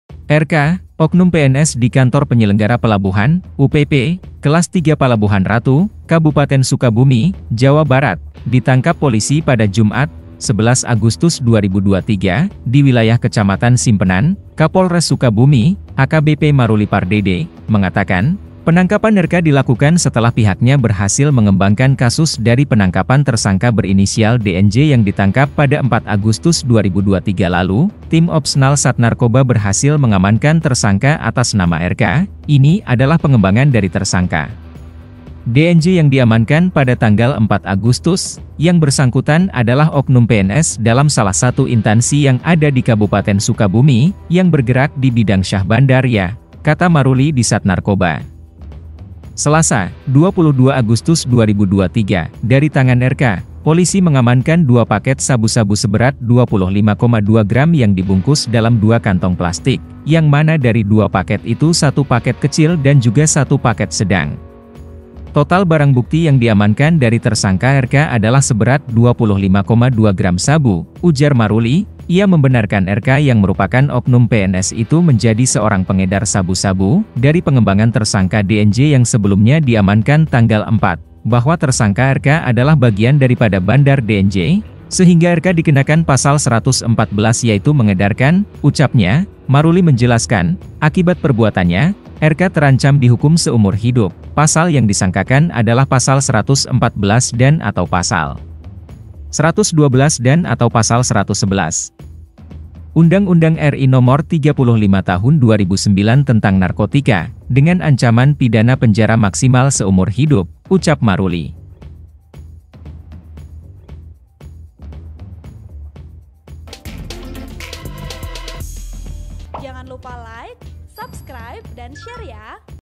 Miliki 25,2 Gram Sabu-sabu, Oknum PNS di Palabuhanratu Sukabumi Ditangkap, Terancam Bui Seumur Hidup
Artikel ini telah tayang di TribunJabar.id dengan judul Miliki 25,2 Gram Sabu-sabu, Oknum PNS di Palabuhanratu Sukabumi Ditangkap, Terancam Bui Seumur Hidup,
Penulis: M RIZAL JALALUDIN | Editor: Darajat Arianto |Video Editor: Uben